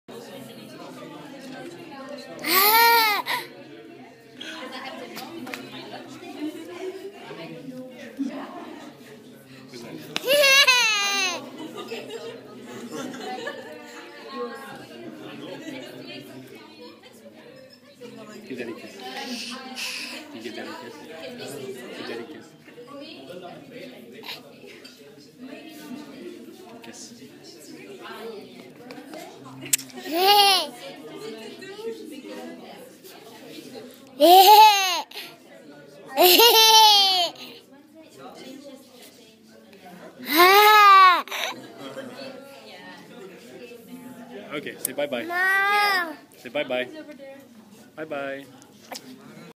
Anouk at the VUB
Anouk plays with the camera, kisses herself on the screen and says bye-bye.